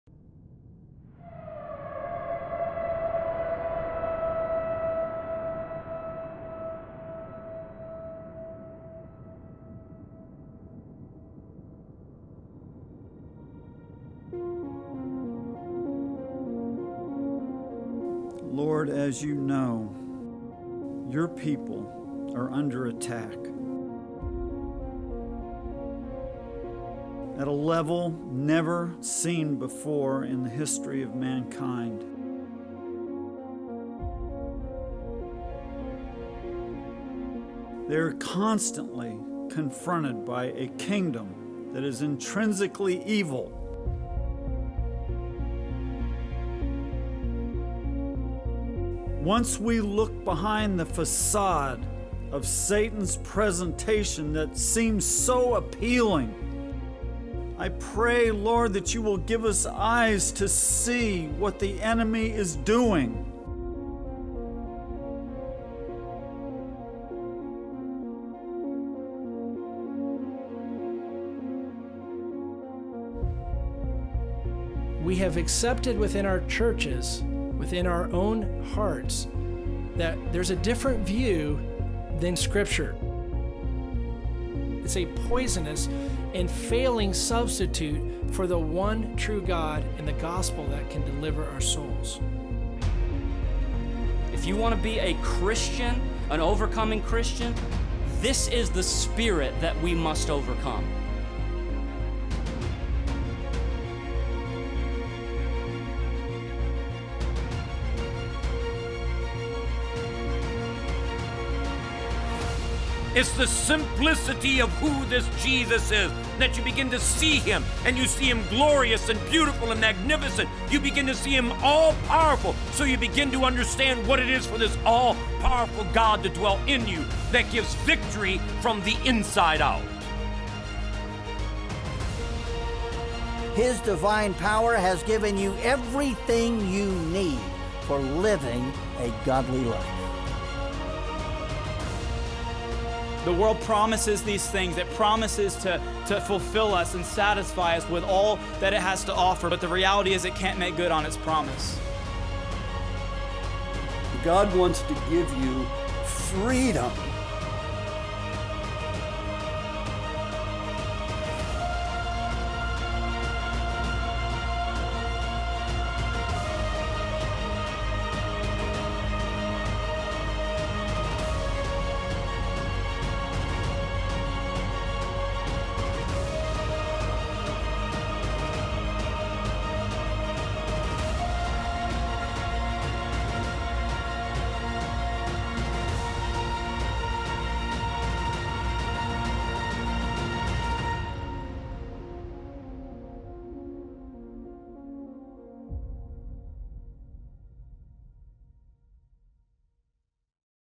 Be an Overcomer! | The Overcomers Series
Our brand-new video resource, "The Overcomers Series", will not only teach you how to lead a godly life but will help you walk in God's power to do so.